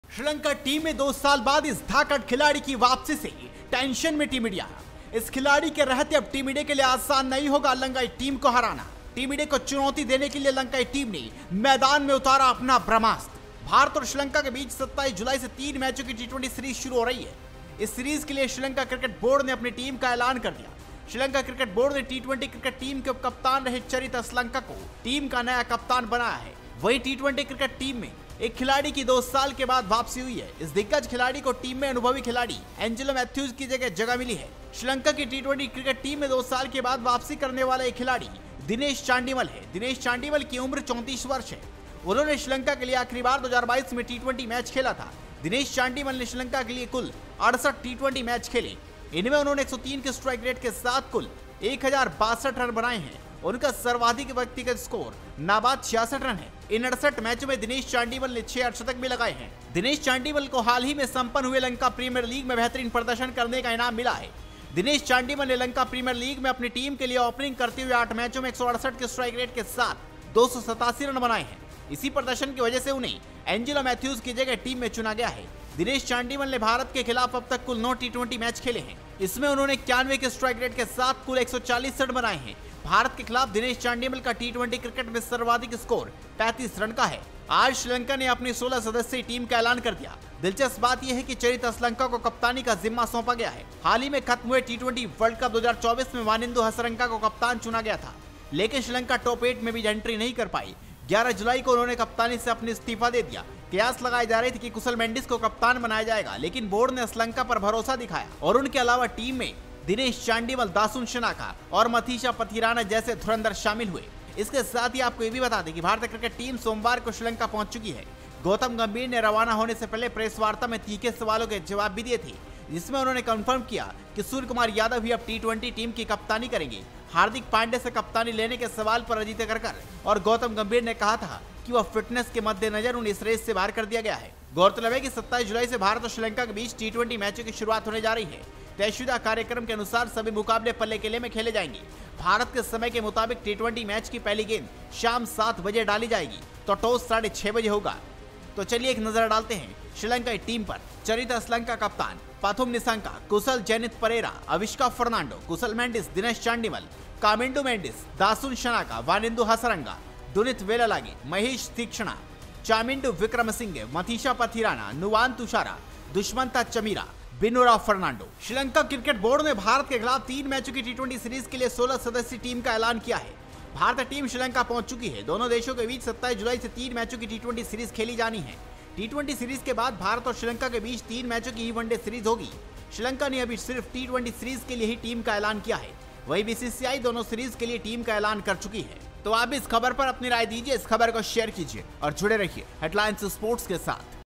श्रीलंका टीम में 2 साल बाद इस धाकड़ खिलाड़ी की वापसी से टेंशन में टीम इंडिया | Headlines Sports
श्रीलंका टीम में 2 साल बाद इस धाकड़ खिलाड़ी की वापसी से टेंशन में टीम इंडिया